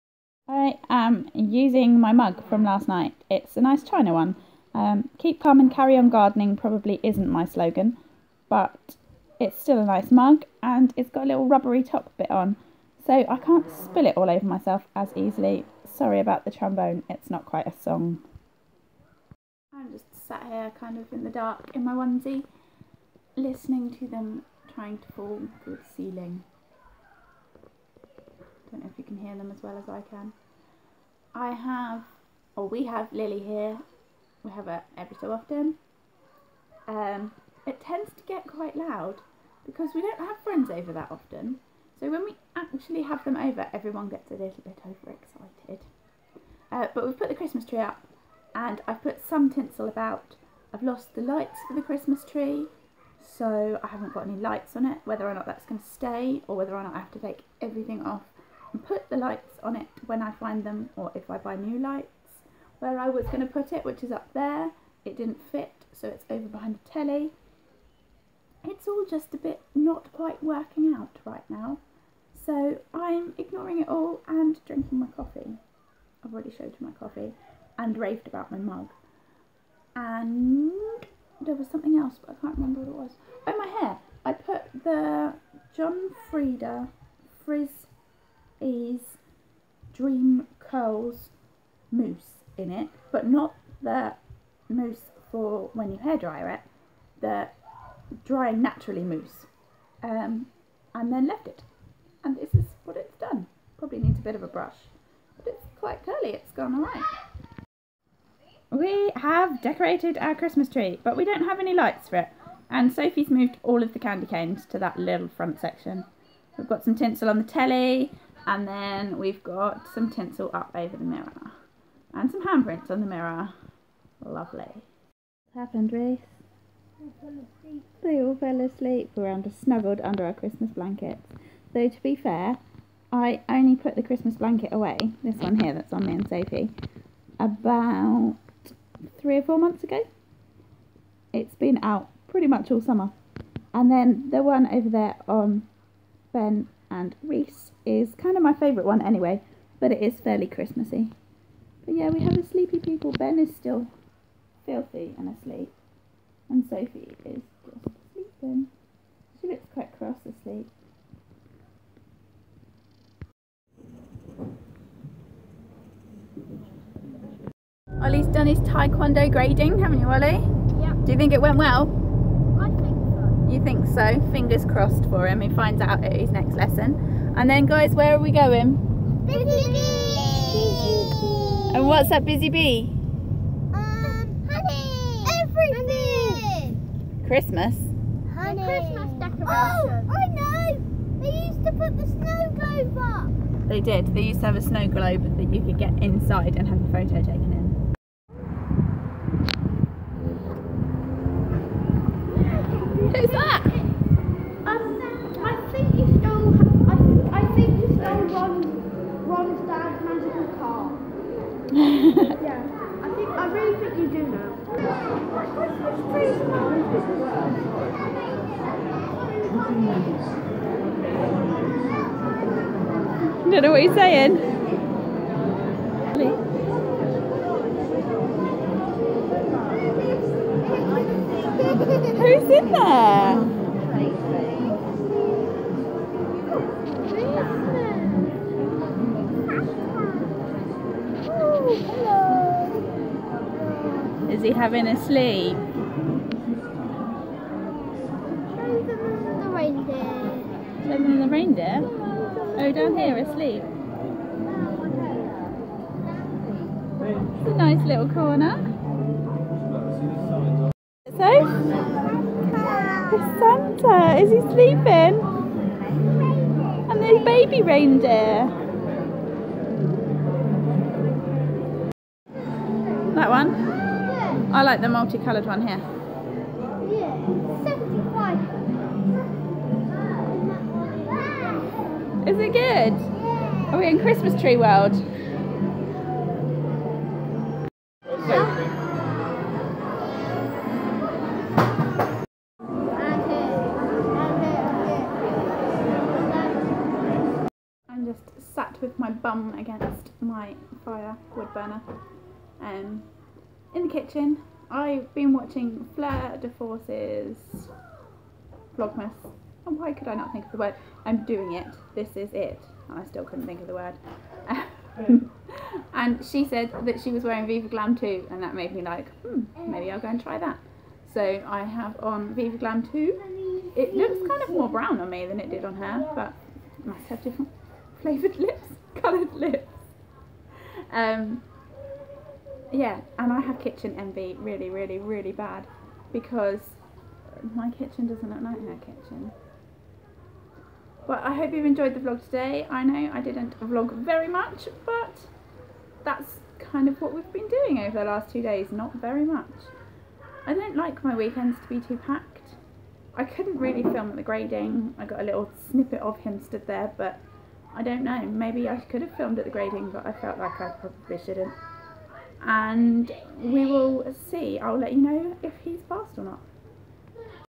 Vlogmas 3 & 4 | Busy Bee
A Vlogmas weekend. We put up our tree and had some fun at Busy Bee, seeing a sleepy Santa after his driving. xx